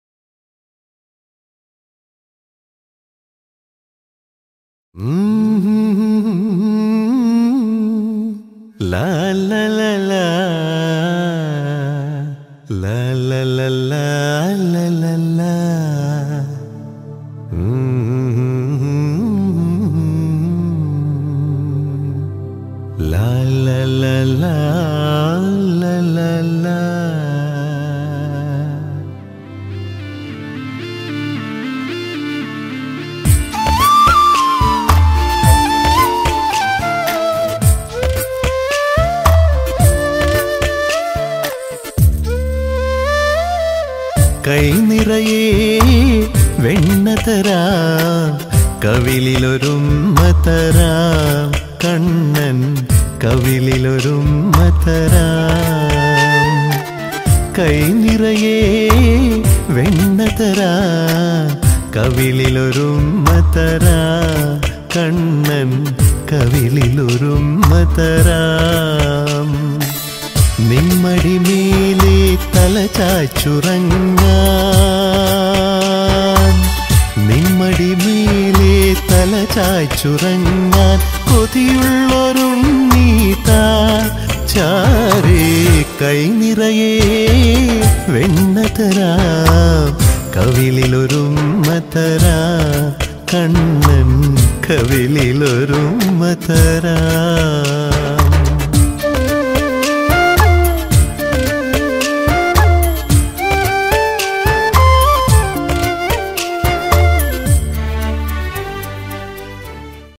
അമ്മയ്ക്ക് ഒരായിരം പിറനാൾ ആശംസകൾ ll Arjun Creations
Rema Ramachandran 60th birthday celebration highlights